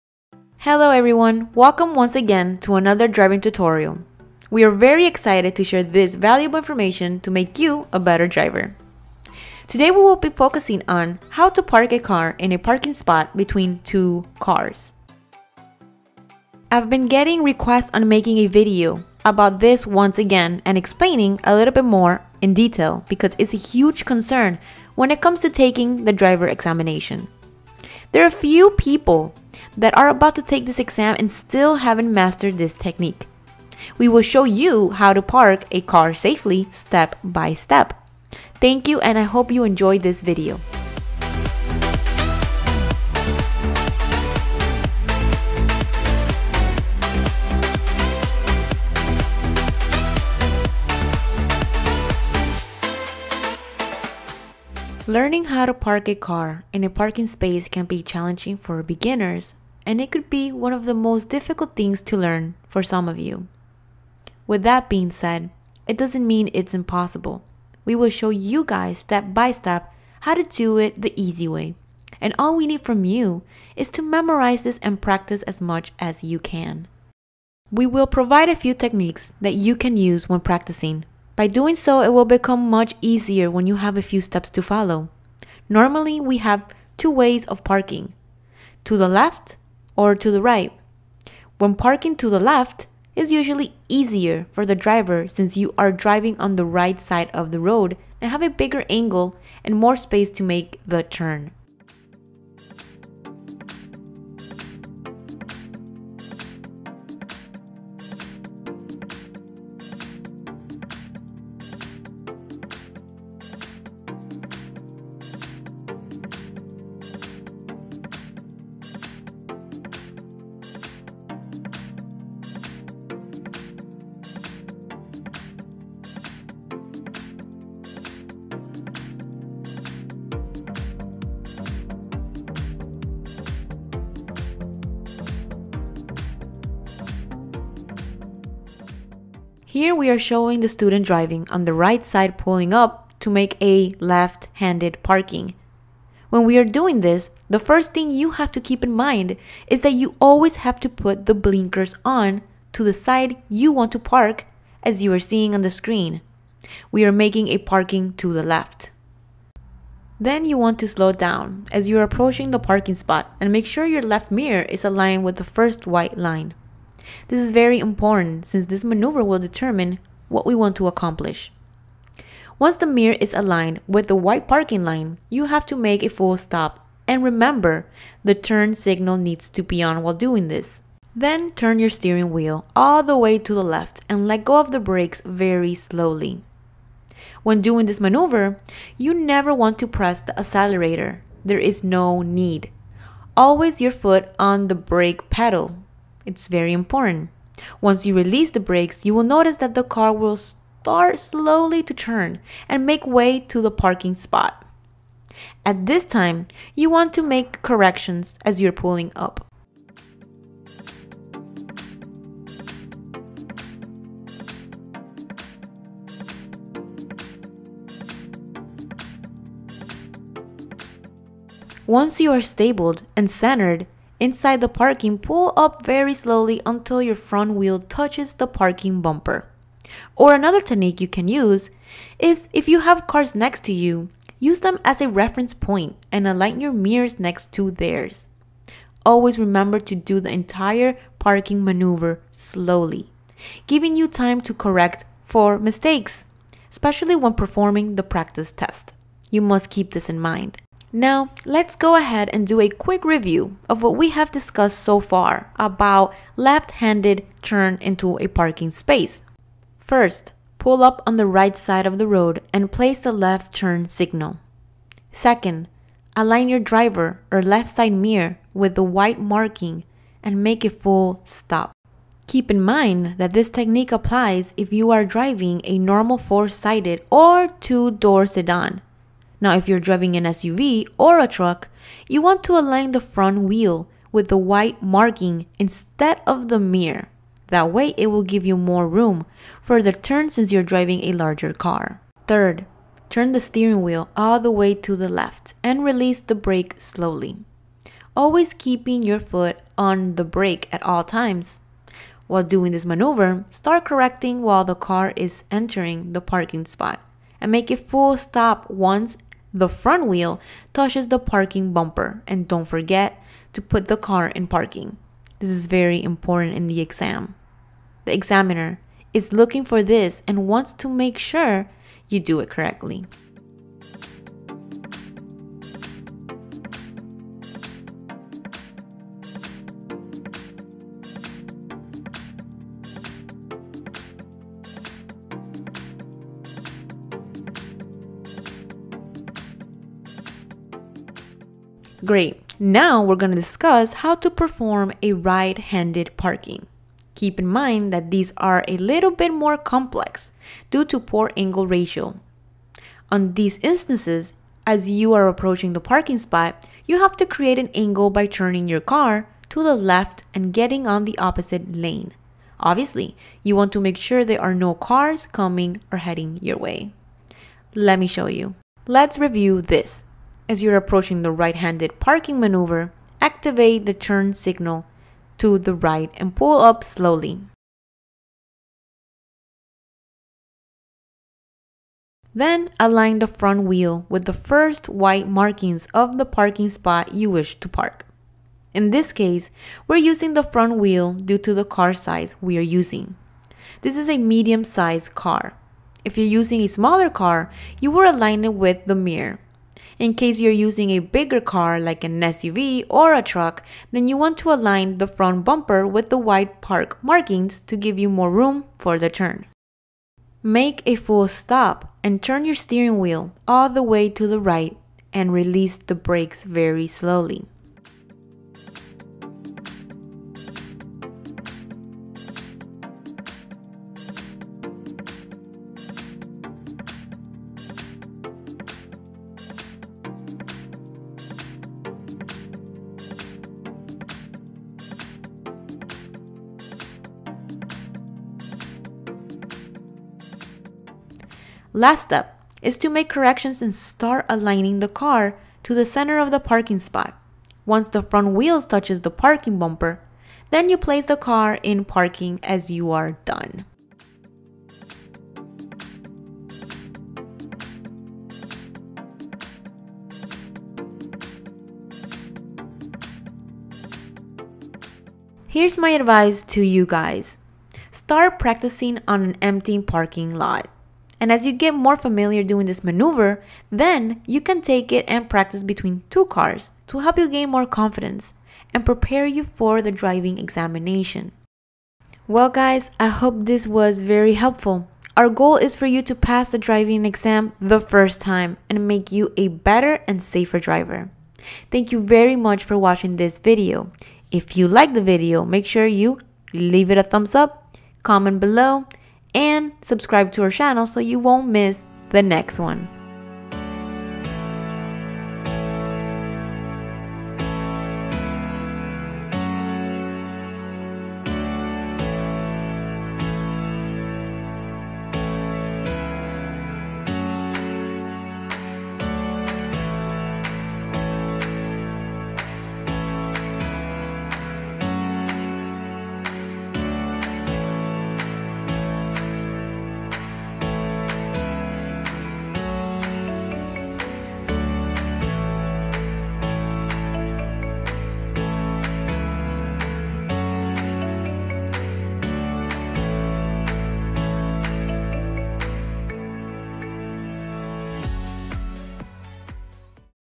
HOW TO PARK IN A PARKING LOT (90 DEGREE PARKING)
Hello everyone!!!, Welcome once again to another driving tutorial and we are very excited to share this valuable information to make you a better driver. Today we will be focusing on how to park your car in a parking lot (These technique is often called perpendicular parking or 90 degree parking. This technique is one that you have to learn regardless, you will be using it for the rest of your life and it is very important you master it. On this video we will teach you how to park a car step by step in a way that will help you remember. For those that have not taken the Road Test this video will help you get thru that as well. Thank you and I hope you enjoy the video.